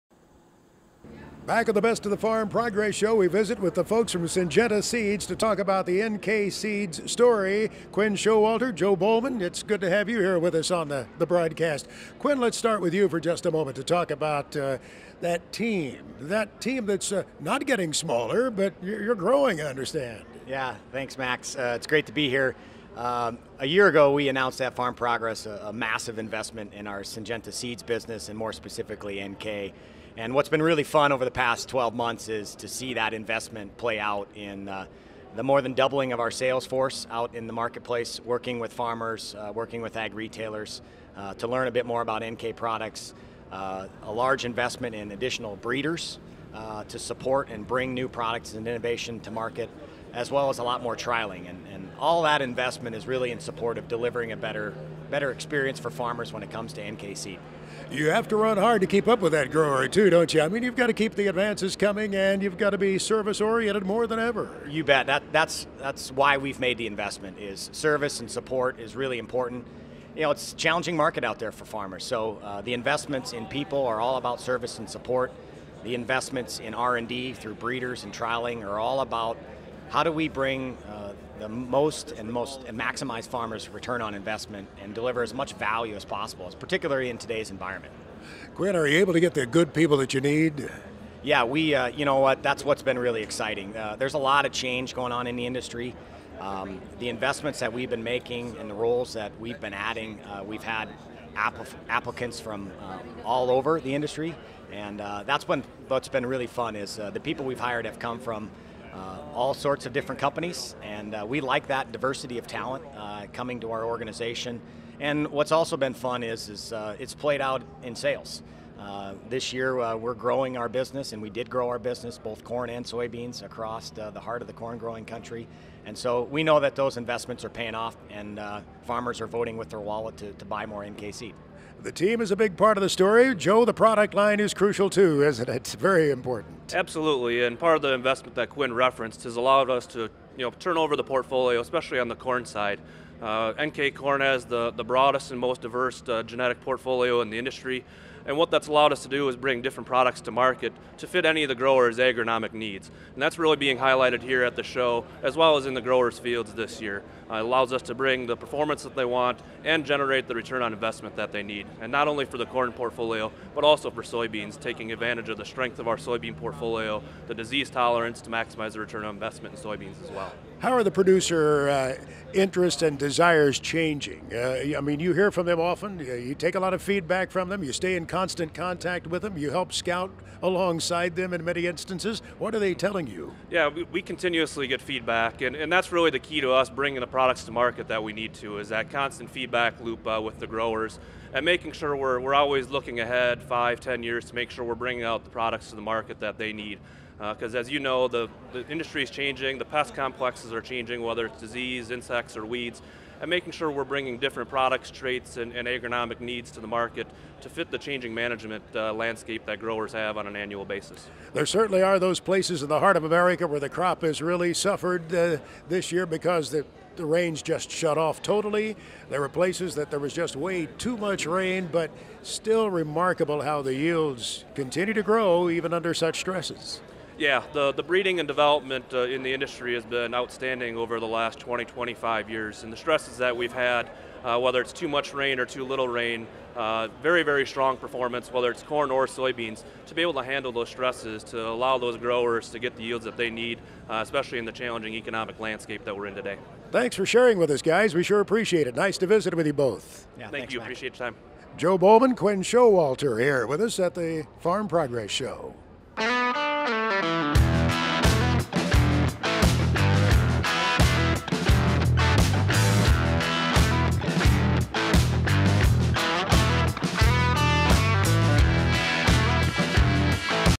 Best of Farm Progress Show 2018 with NK
Join Quinn Showalter, head of sales for NK Seeds, and Joe Bollman, NK corn product manager, as they talk to RFD-TV about the innovation behind NK and how recent investments are benefiting farmers.